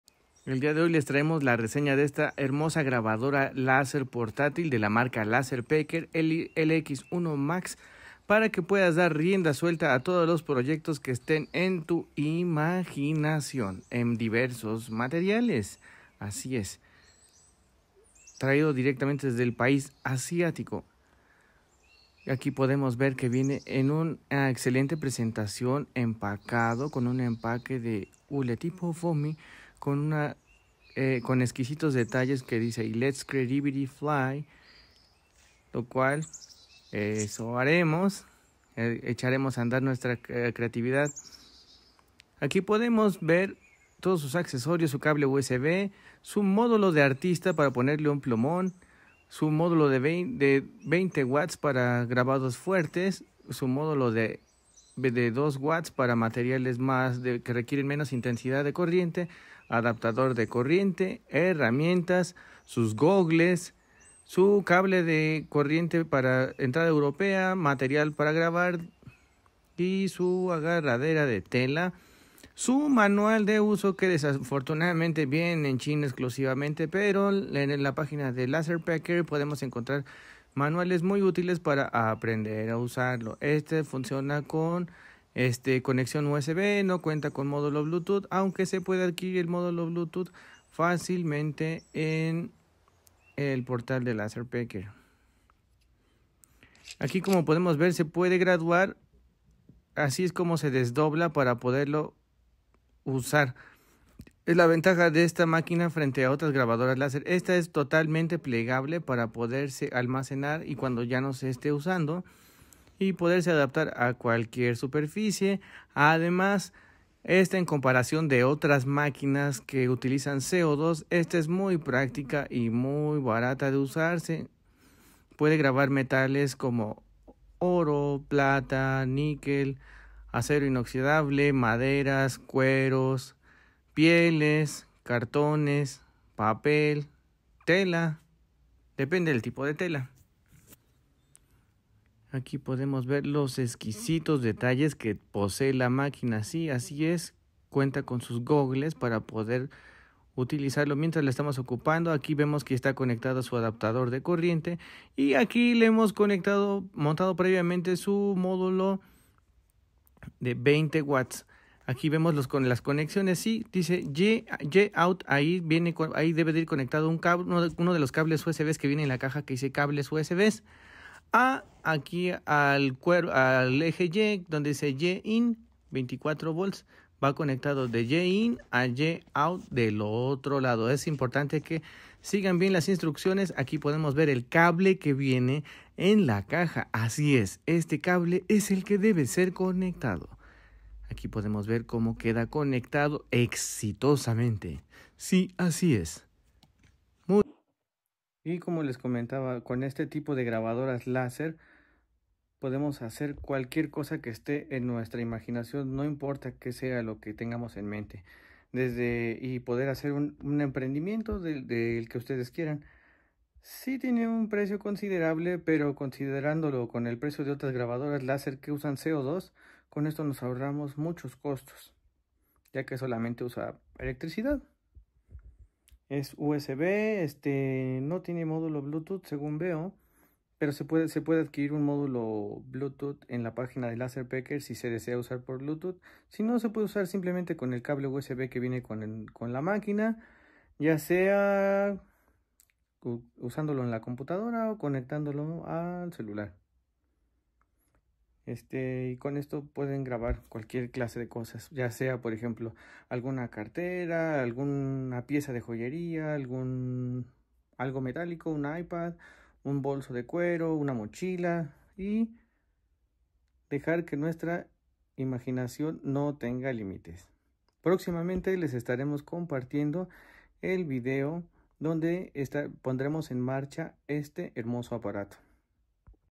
Unboxing de la nueva grabadora láser LaserPecker LX1 Max
La nueva grabadora láser, marca LaserPecker LX1 Max, puede grabar metales como acero inoxidable, oro, plata; también puede grabar vidrio, cartón, madera, cuero, papel, etc.

La herramienta ideal para el taller, para poder hacer tus sueños realidad.